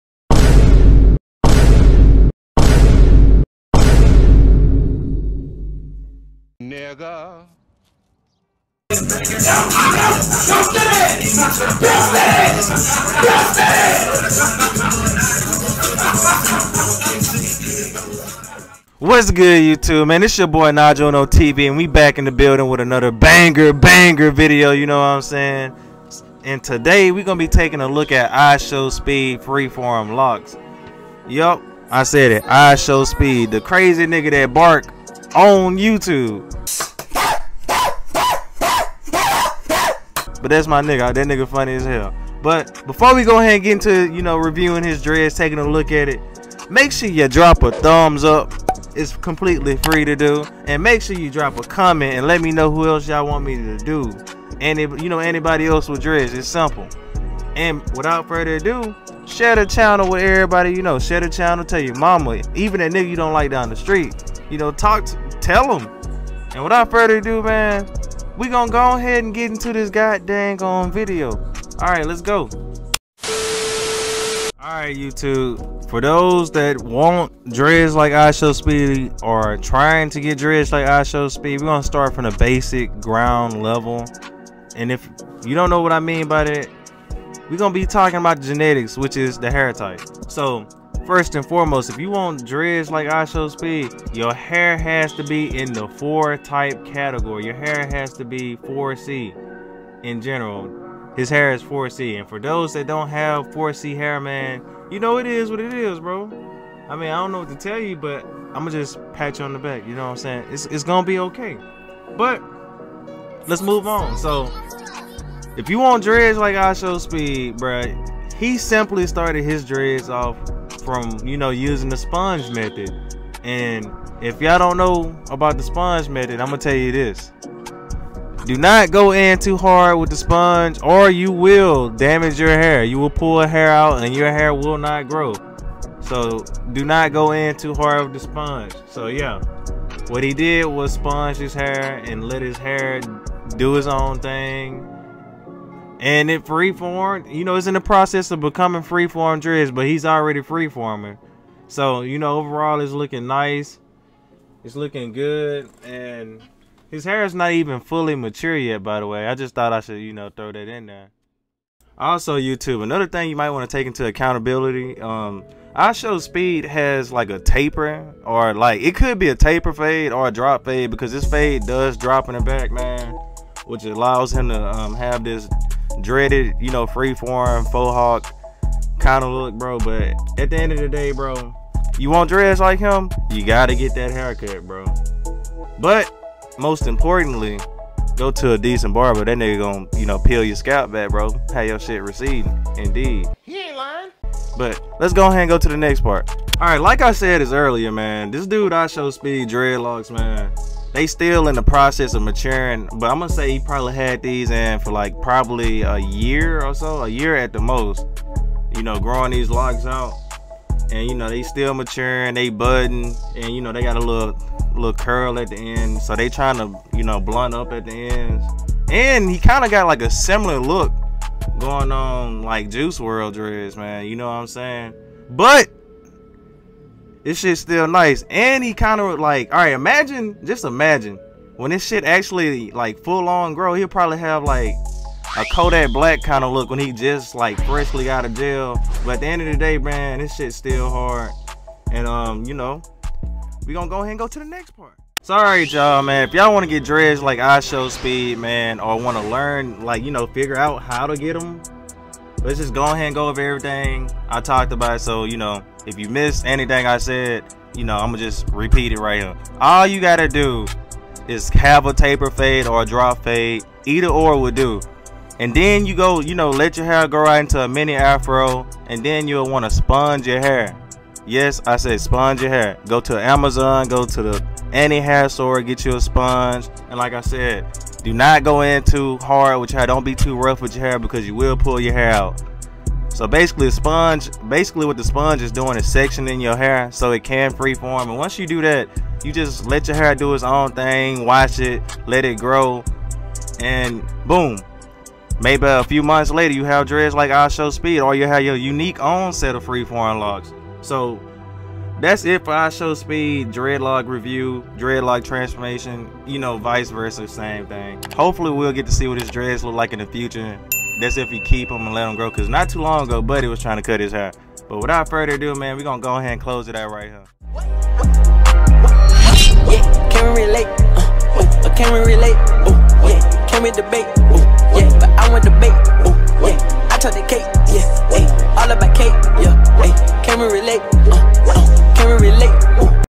ISHOWSPEED FREEFORM LOCS* HOW TO GET FREEFORM DREADS LIKE ISHOWSPEED*
IN THIS VIDEO I REVIEW ISHOWSPEED FREEFORM DREADS AND TELL YOU HOW OBTAIN DREADS LIKE HIM.

2.LIKE THIS VIDEO
4.SHARE THE VIDEO

TURN THE NOTIFICATIONS BELL 🔔 ON‼️‼️‼️‼️

✅PSN🎮|THETITTIETICKIER✅